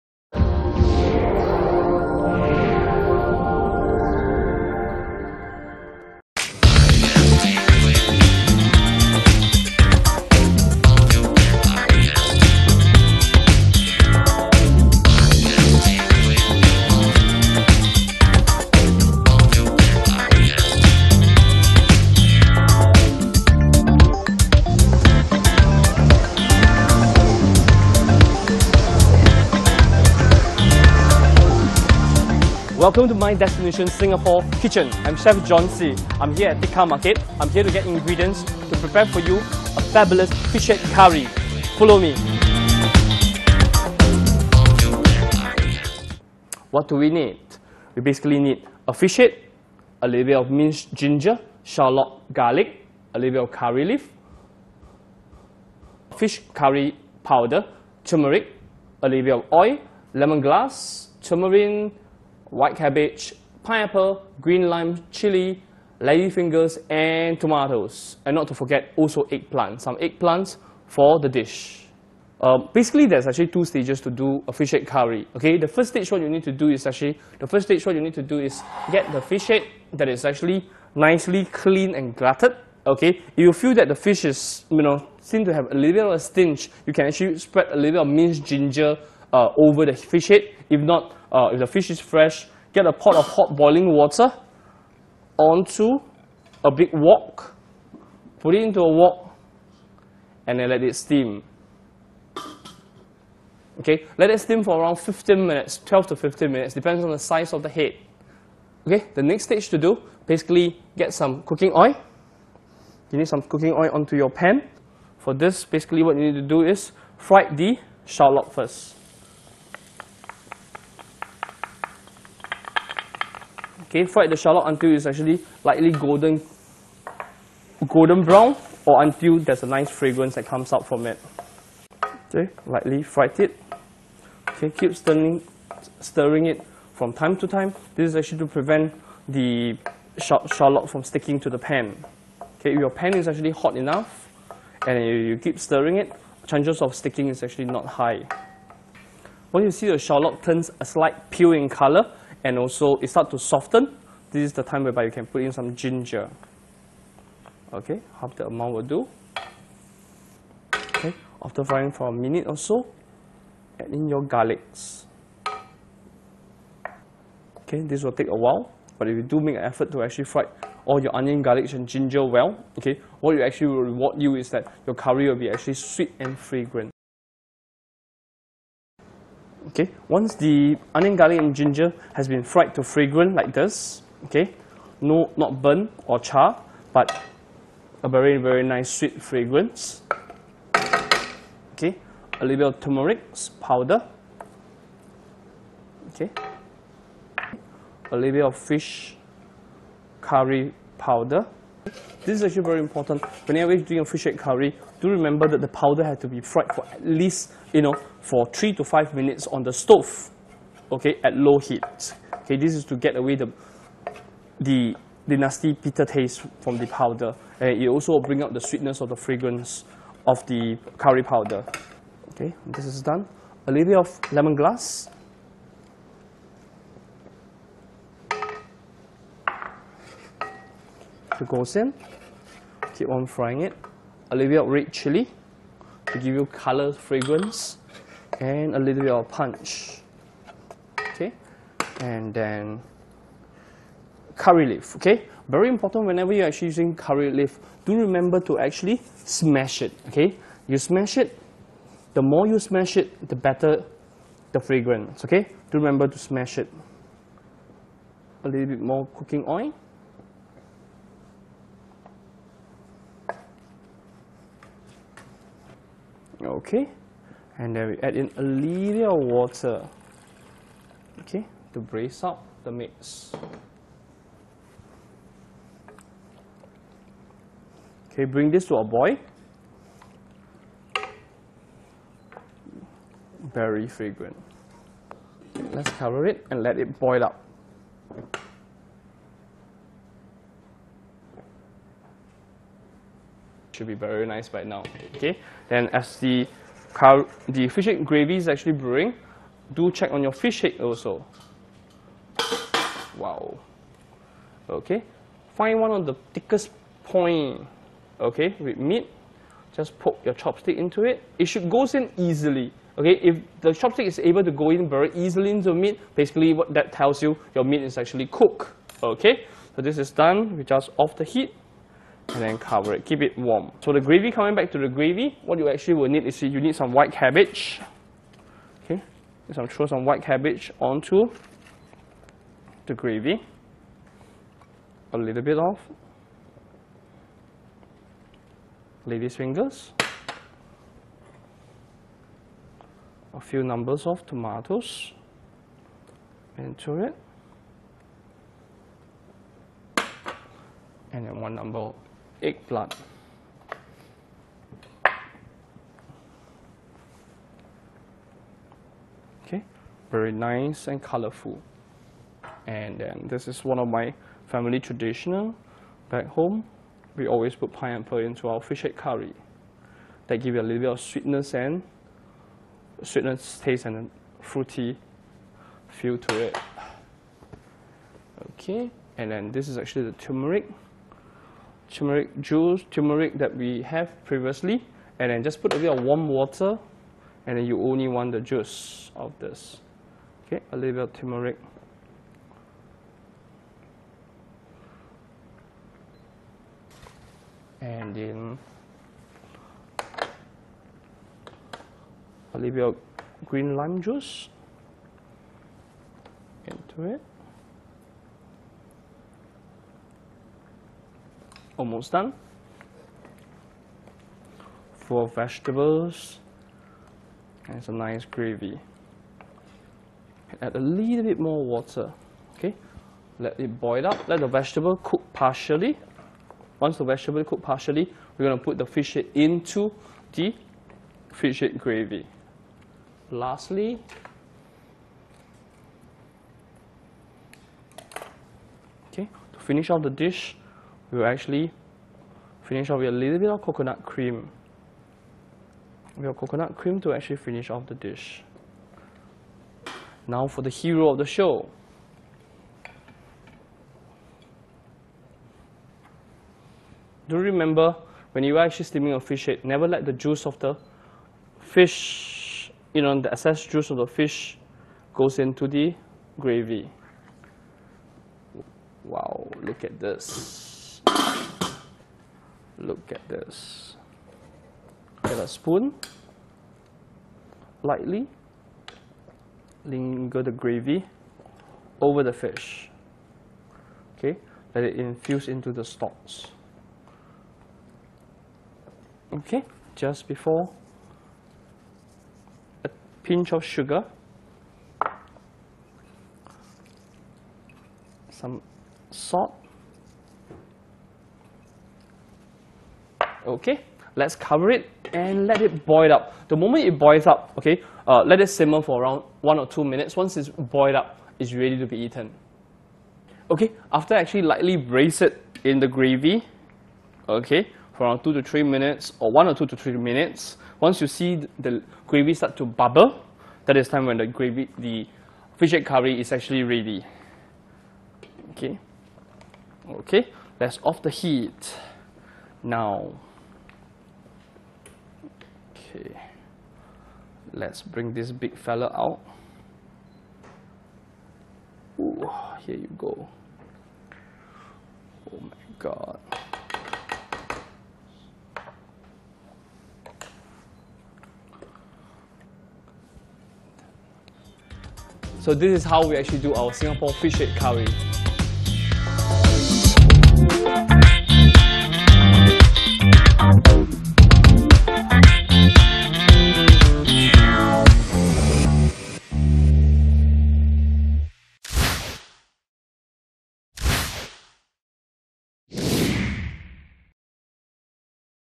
My Destination Singapore Kitchen - Fish Head Curry
With My Destination Singapore Kitchen, learn how to cook Singapore's fabulous traditional dish 'Fish Head Curry' with this easy how to video.